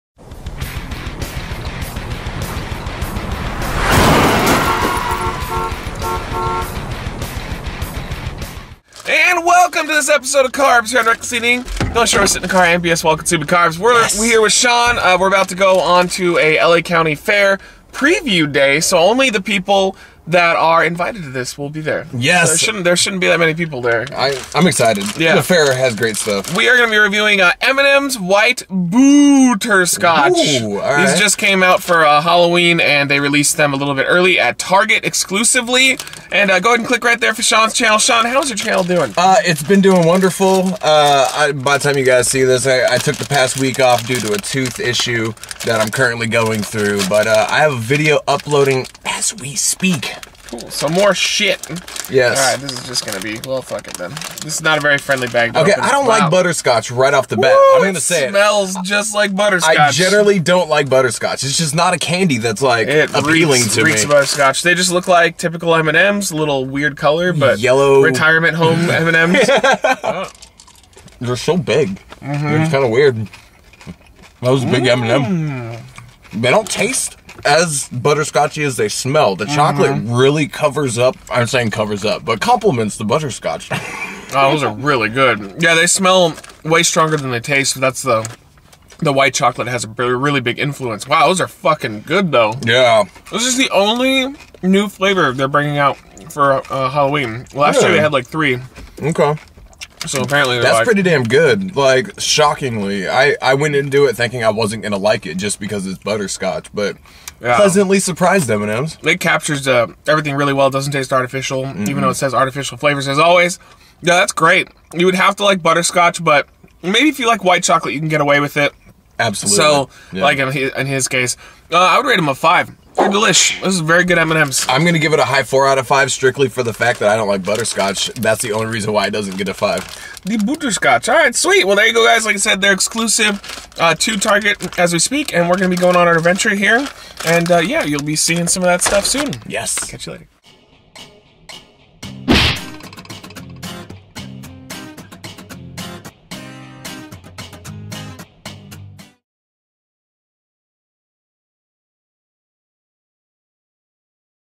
M&M's White BOO-tterscotch Review - CarBS
Welcome to CarBS! On this show Matt Zion will be reviewing food/drinks in his car to get them out to the fans asap! Today Zion reviews M&M's White BOO-tterscotch!